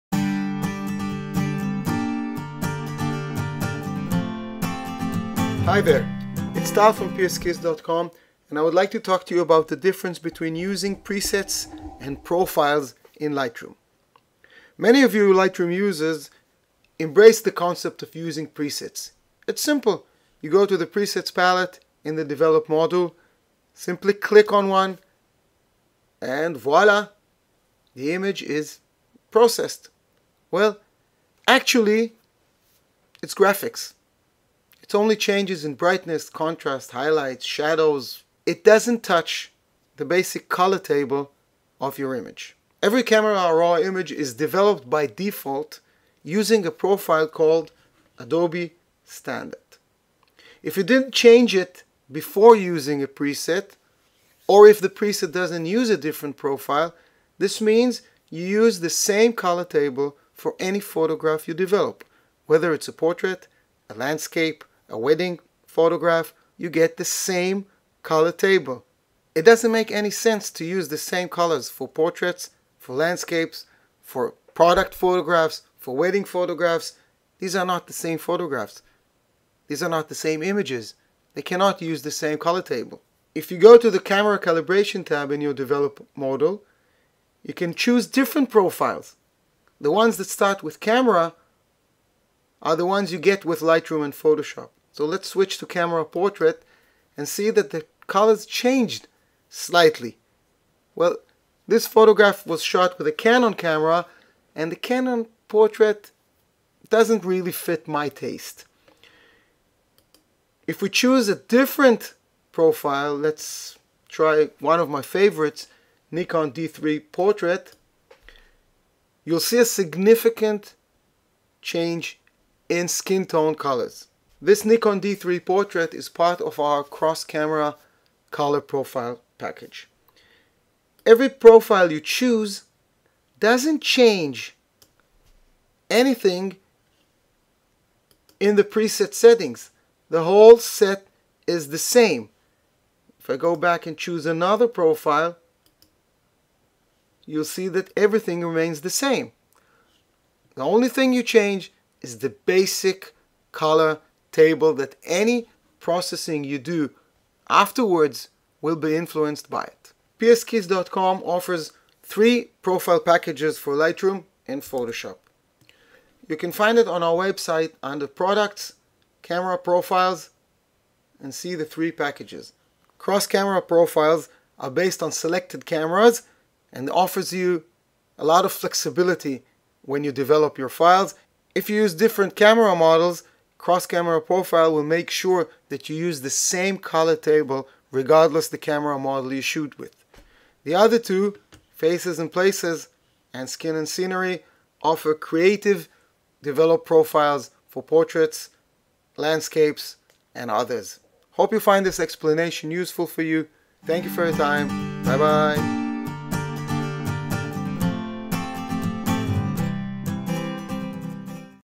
Profiles vs Presets in Lightroom
The basic differences between using profiles or presets in Lightroom (and Photoshop) and the profiles advantageד.